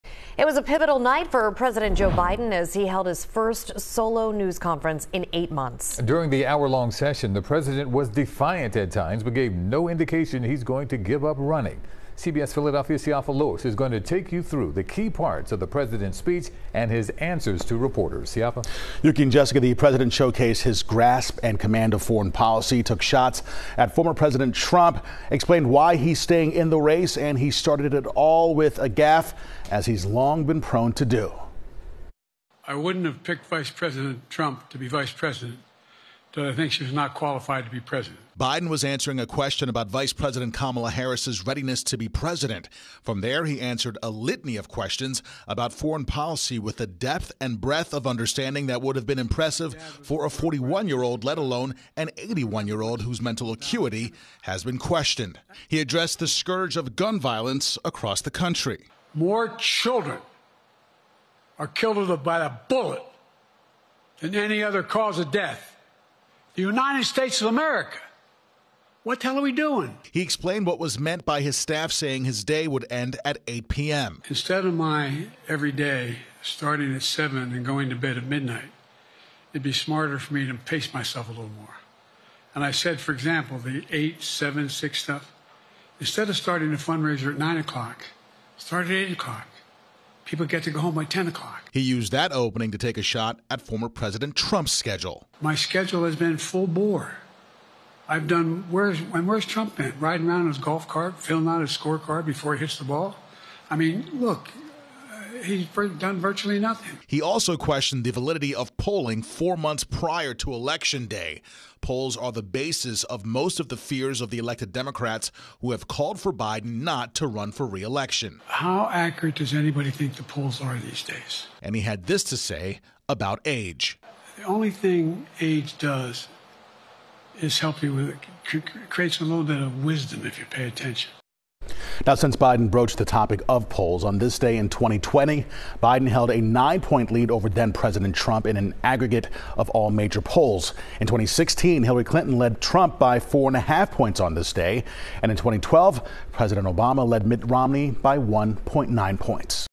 Joe Biden addresses doubts about his campaign during news conference
President Joe Biden was defiant at times but gave no indication he intends to give up running for re-election during a news conference Thursday night.